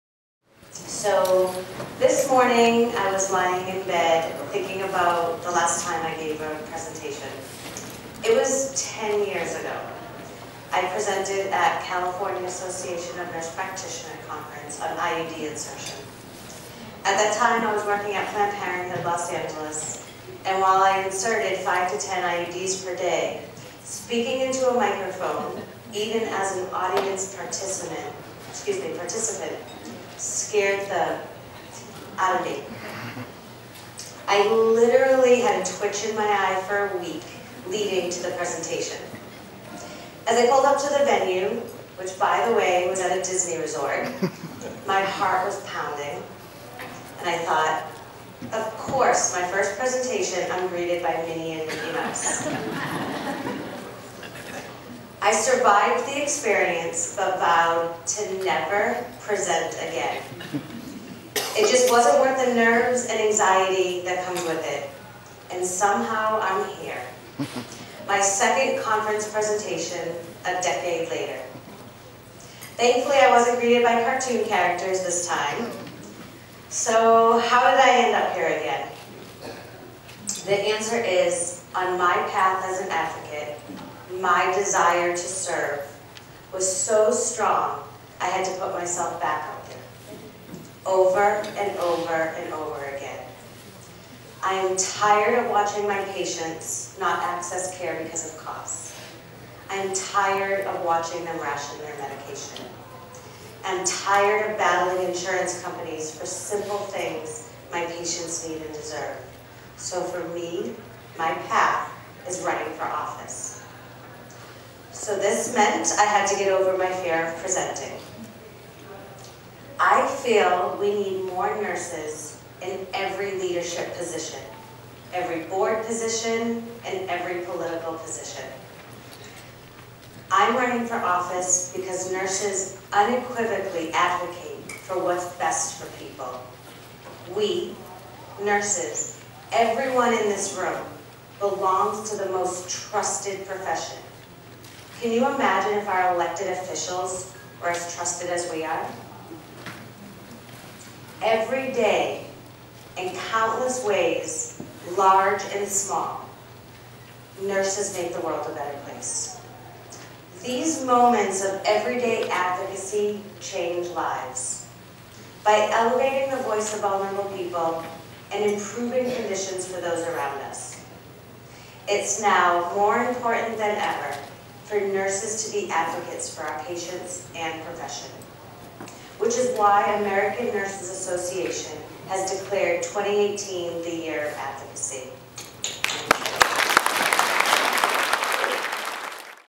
Nursing & Advocacy
Rachel's keynote address at the 2018 ONA conference.